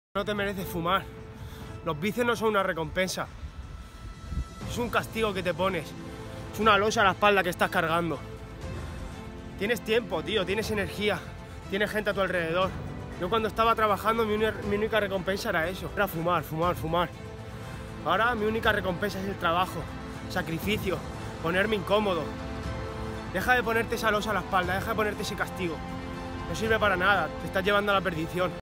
❗LOS VICIOS NO SON UNA RECOMPENSA
Durante muchos años el motor de mi vida habían sido los vicios; porros, tabaco, alcohol...

No podía ir a ningún sitio si no era con un porro encima, no podía comer sin haber fumado antes, no podía viajar, hacer ocio... sin la sustancia.

Ahora, mi recompensa es el esfuerzo, la disciplina, el desarrollo personal y aportar valor desde mi experiencia a los demás.

Nunca te conformes con tener un "pequeño" vicio, siempre hay algo detrás de eso.